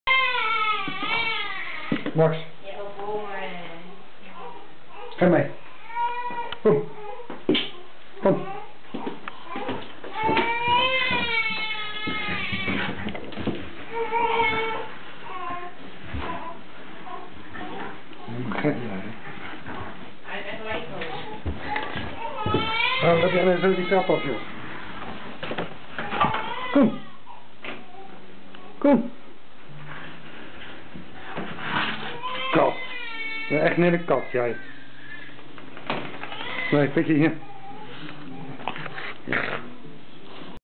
Franse bull MAX traplopen
De franse bull die heel vreemd een trap afloopt (als een kat)